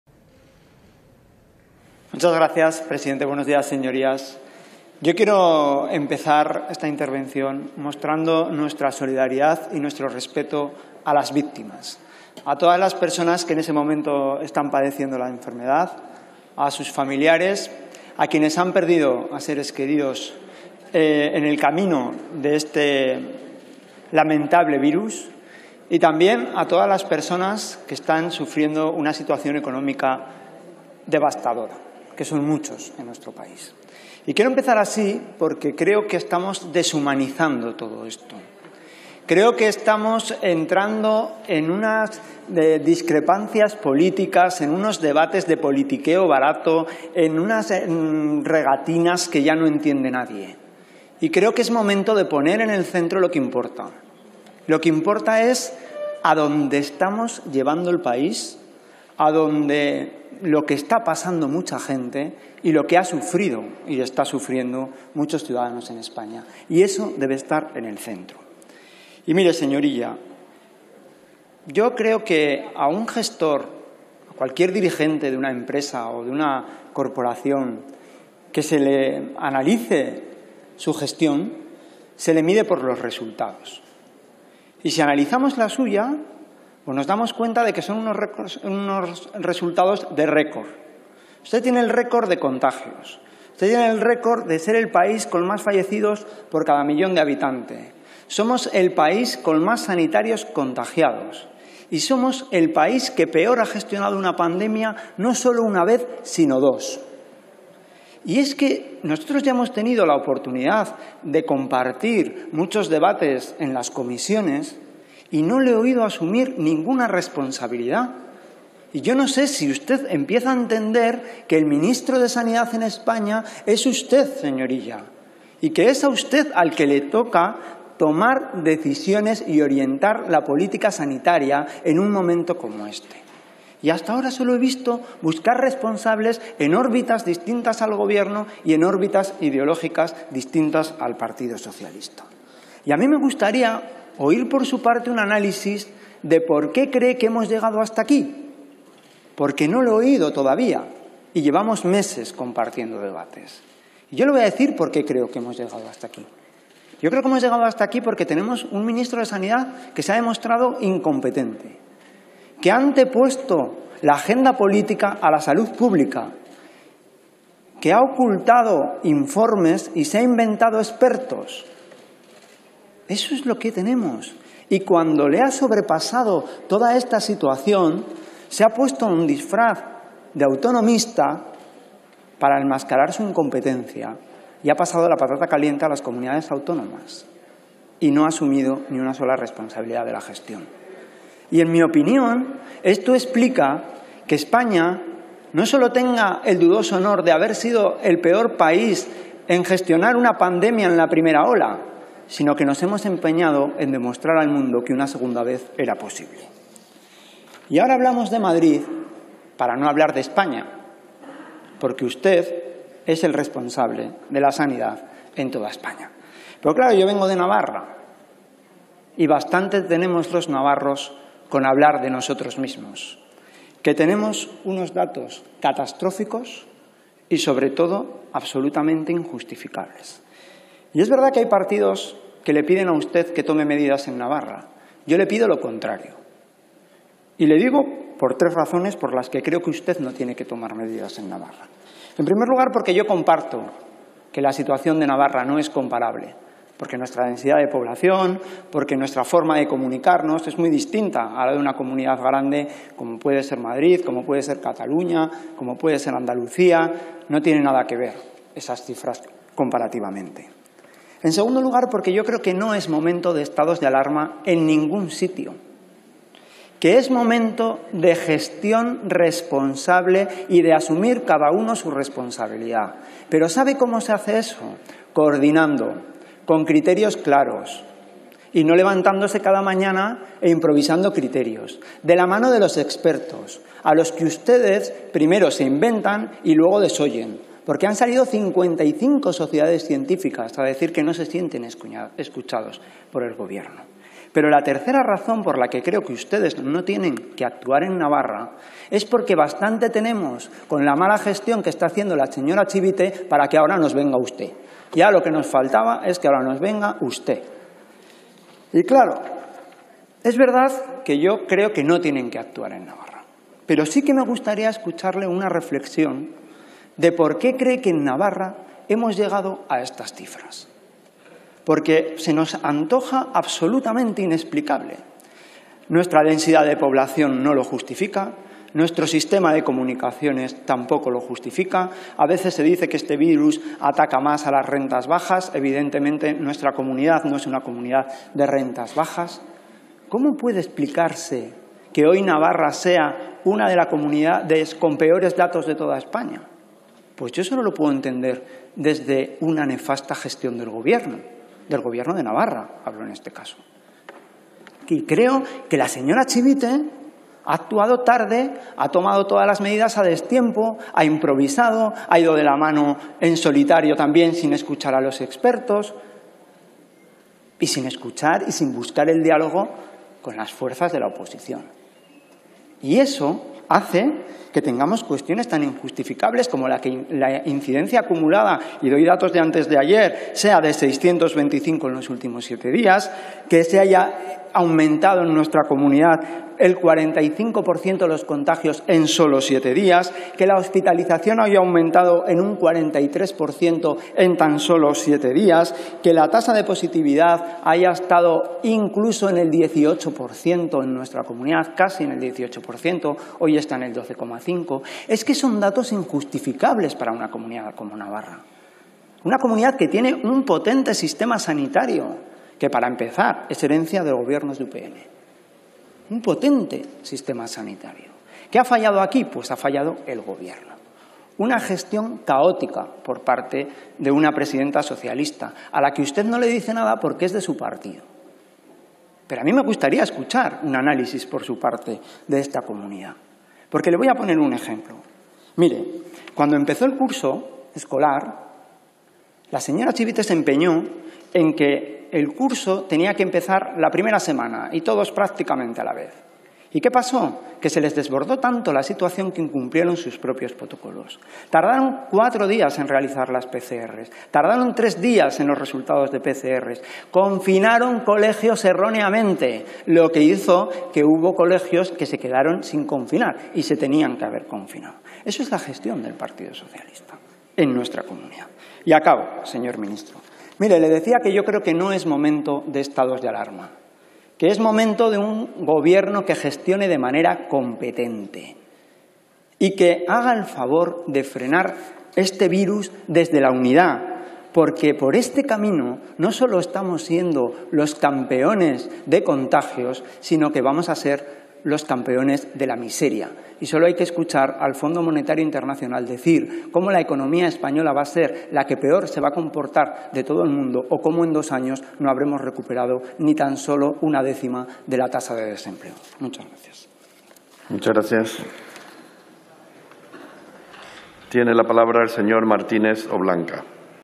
Intervención de Sergio Sayas hoy 15 de Octubre 2020 sobre estado de alarma en Madrid
Intervención de Sergio Sayas López de Navarra Suma, en la Comunicación del Gobierno que acompaña al Real Decreto 900/2020, de 9 de octubre, por el que se declara el estado de alarma para responder ante situaciones de especial riesgo por transmisión no controlada de infecciones causadas por el SARS-CoV-2, el día 15 de Octubre de 2020